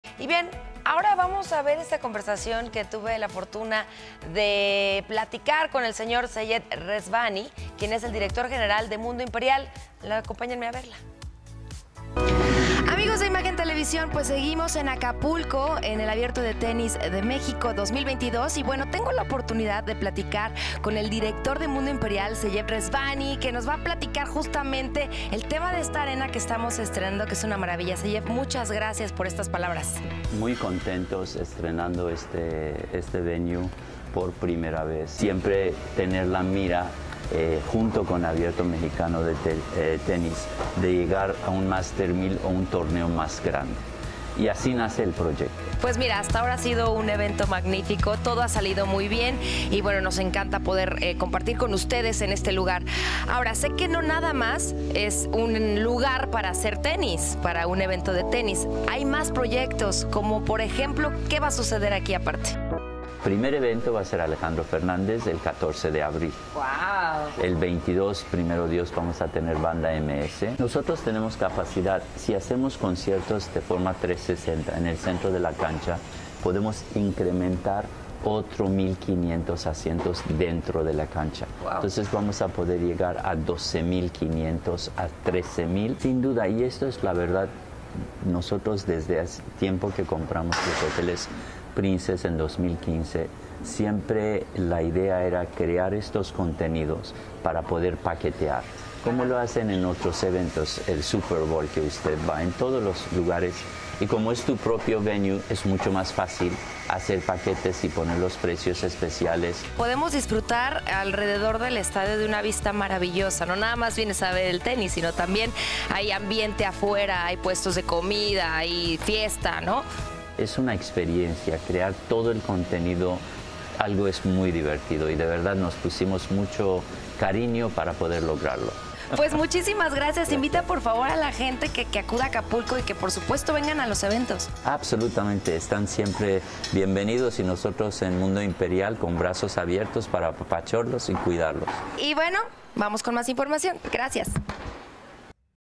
La nueva casa del Abierto Mexicano de Tenis está en Acapulco | Noticias con Francisco Zea
Seyed Rezvani, director de Mundo Imperial, habla del proyecto y diseño de la arena donde se llevó a cabo el Abierto Mexicano de Tenis 2022.